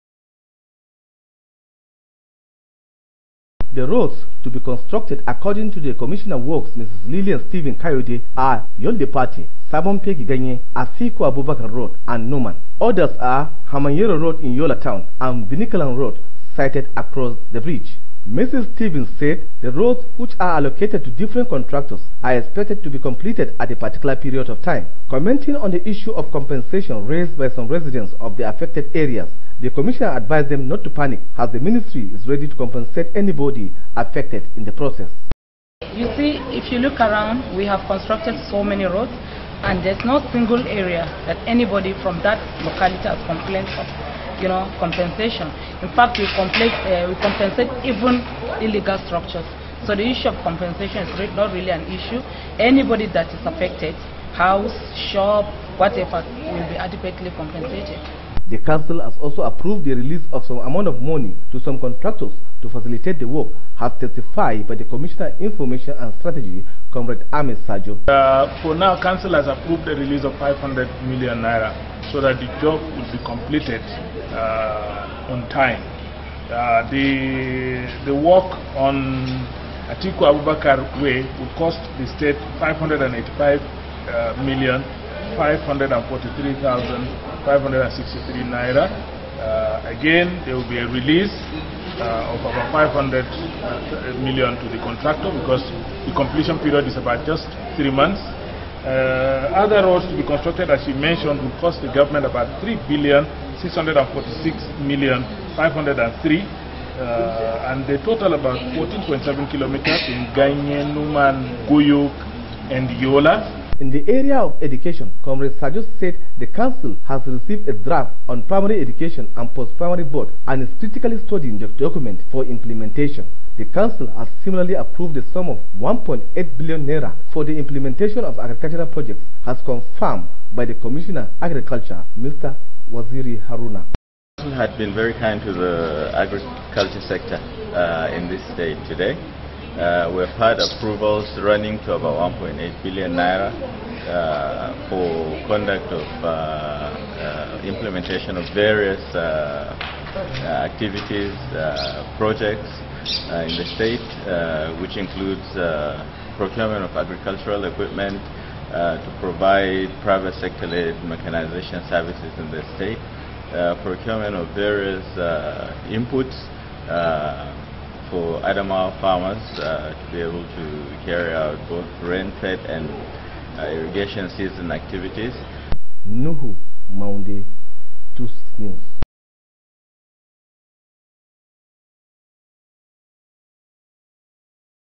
EXCO MEETING REN
The State Government has approved the construction of eight roads located within and outside the State.